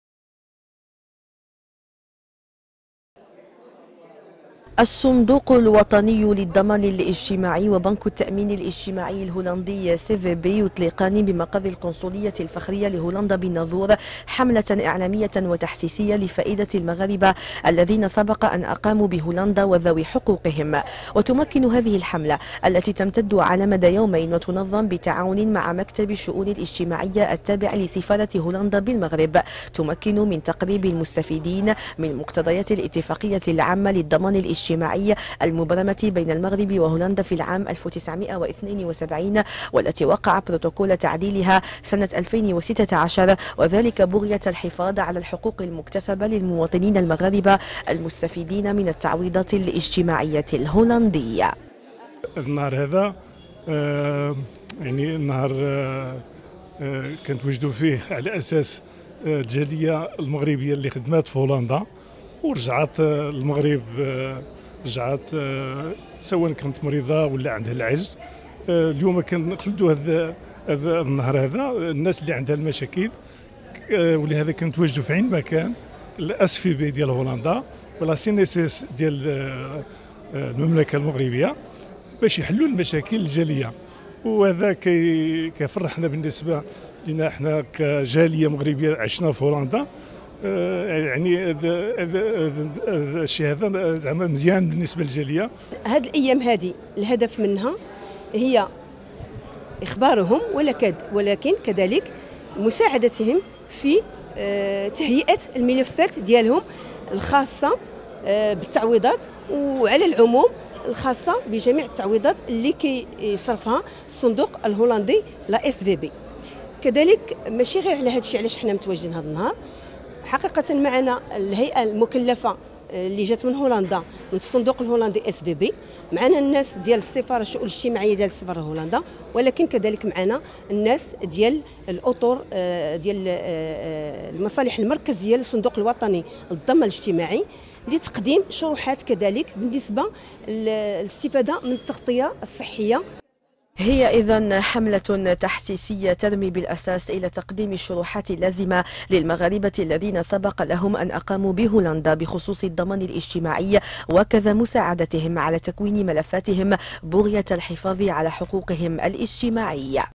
الناظور.. إطلاق حملة تحسيسية حول الحقوق الاجتماعية للمغاربة الذين سبق أن أقاموا في هولندا
أطلق الصندوق الوطني للضمان الاجتماعي وبنك التأمين الاجتماعي الهولندي (سي في بي)، الأربعاء الماضي بمقر القنصلية الفخرية لهولندا بالناظور، حملة .إعلامية وتحسيسية لفائدة المغاربة الذين سبق أن أقاموا بهولندا وذوي حقوقهم